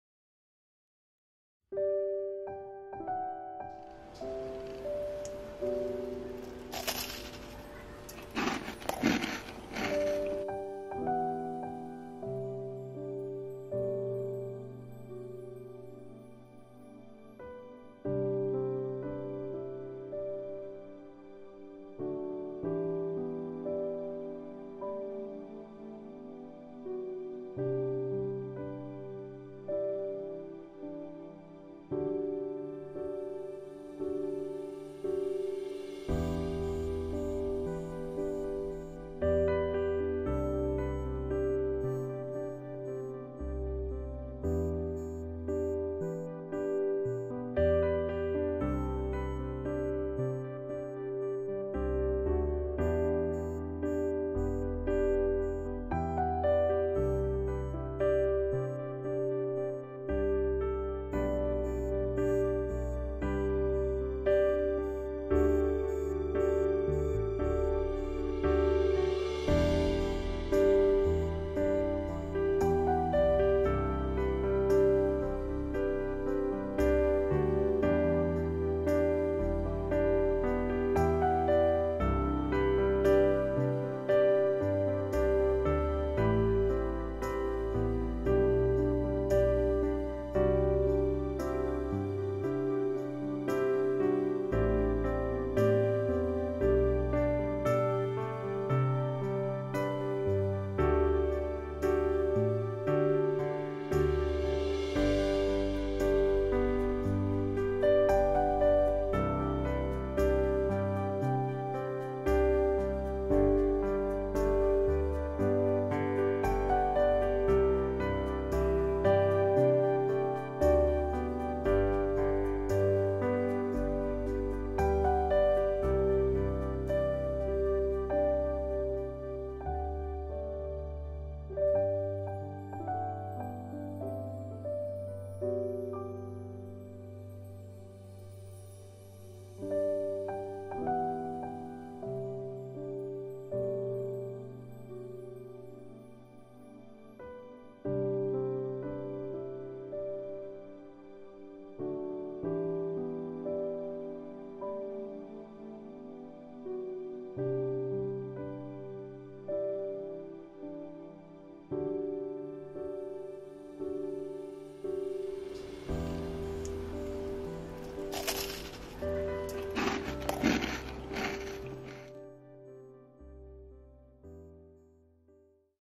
[ENG SUB] 4 ingredients parsley and garlic bread stick recipe, for leftover bread crust🌱超脆欧芹香蒜烤面包🌱
If you have some leftover bread crusts, this is the recipe that you are looking for. In this way, you will not waste any bread crusts and you can enjoy this super crunchy and tasty roast bread~

🌱Ingredients:

bread crusts from 10 pieces of bread
100g olive oil
8g dried parsley flakes
10-15g garlic salt

这个菜谱十分适合烹饪剩余的面包边，这样大家就不用浪费食物啦～脆脆的香香的，又很快手，真的可以充当小零食吃哦～

感谢大家的观看，希望大家也能做起来哦～

🌱材料:

10个面包片的面包边
100克 橄榄油
8克 干欧芹碎
10-15克 蒜香盐

🎹Music🎹